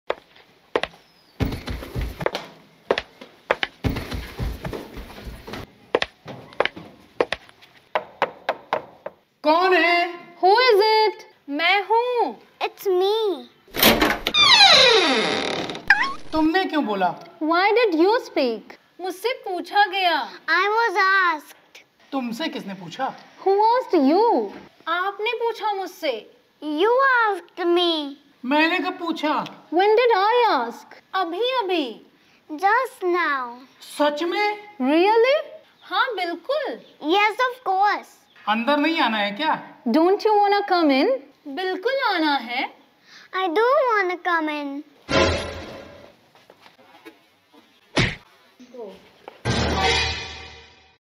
कौन है? मैं हूँ.. English Conversation, 1-Minute English Speaking Practice, Kanchan Keshari
In this 1-minute English learning shorts video, you’ll learn daily use English conversation by Kanchan Keshari Ma’am.